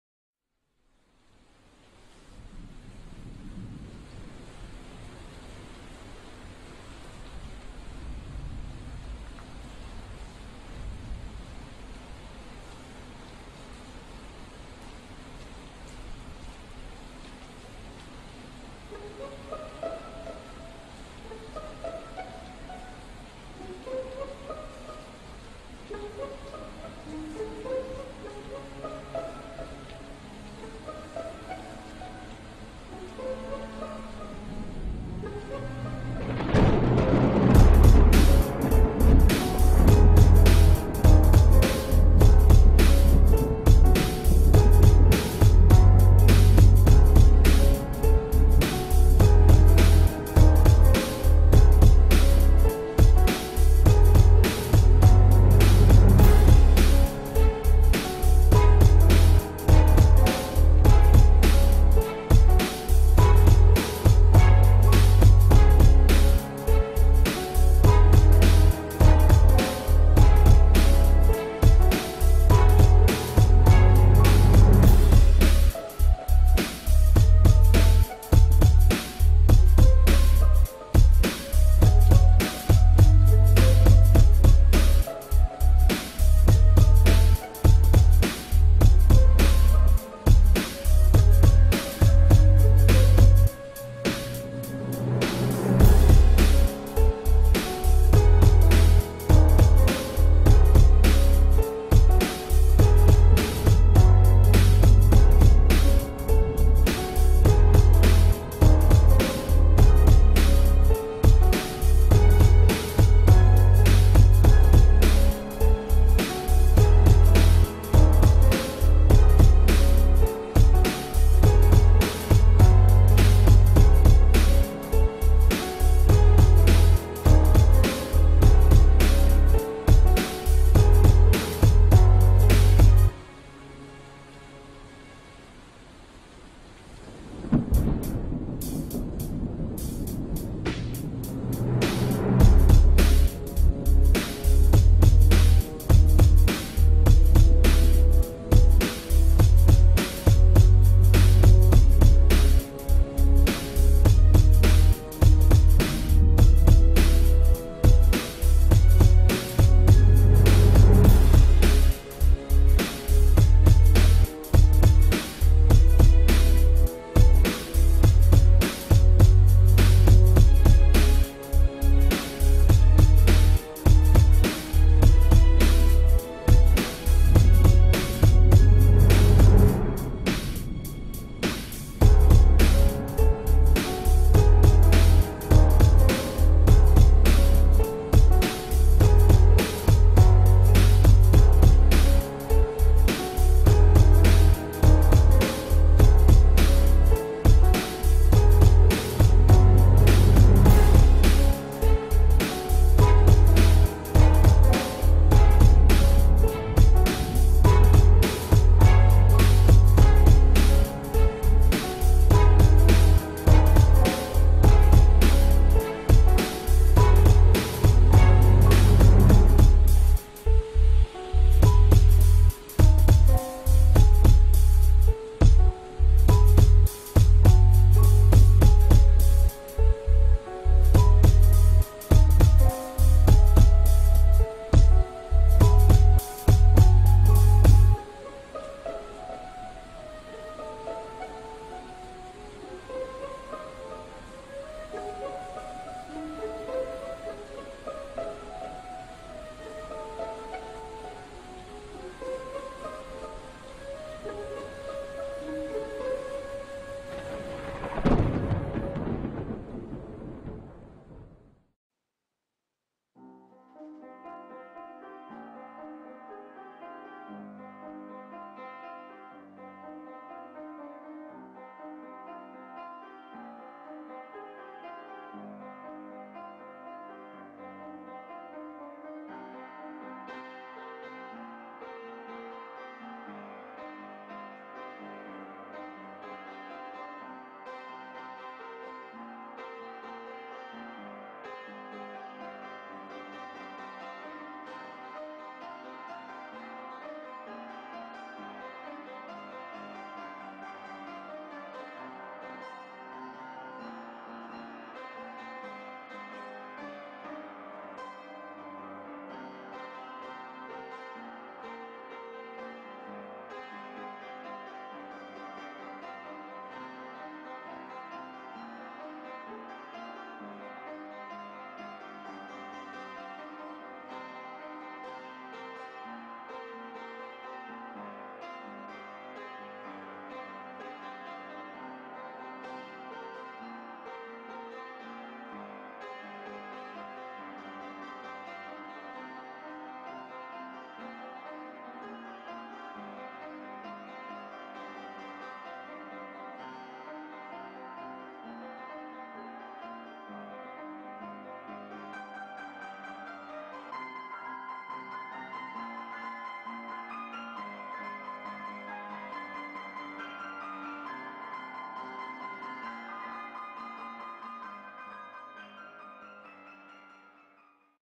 'CHANGE' - A Speed-Drawing Film. || ROY COPPER
This was a class project I was assigned, and I guess I just ran with it.